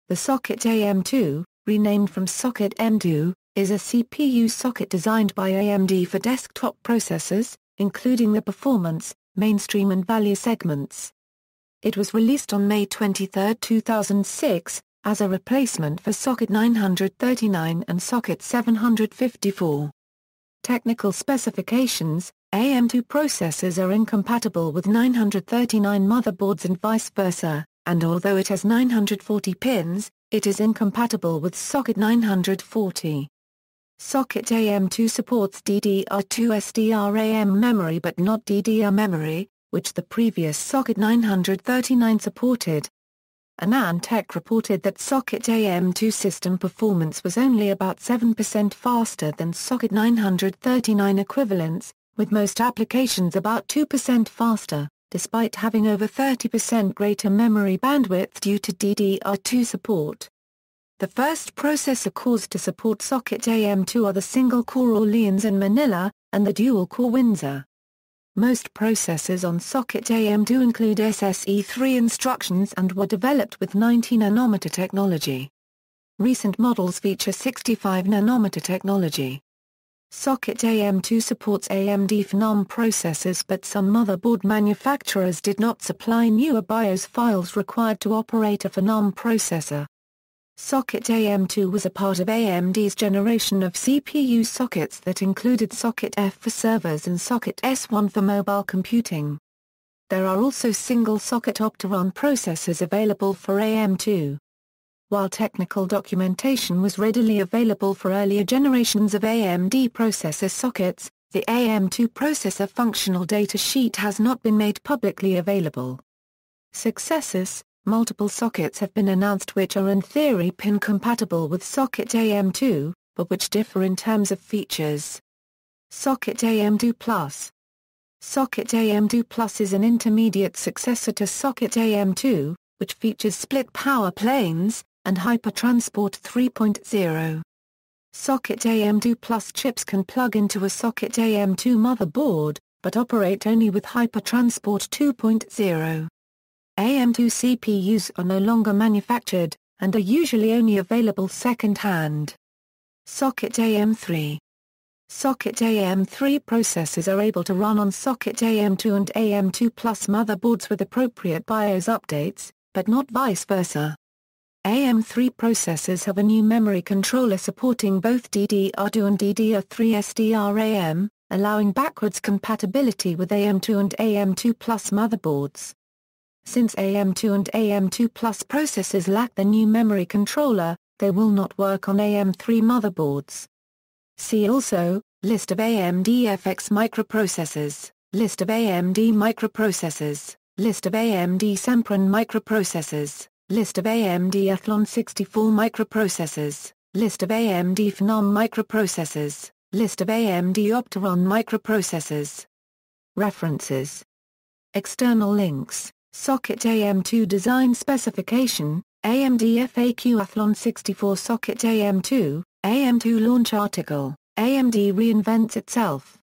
Socket AM2
The Socket AM2, renamed from Socket M2, is a CPU socket designed by AMD for desktop processors, including the performance, mainstream and value segments. It was released on May 23, 2006, as a replacement for Socket 939 & Socket 754.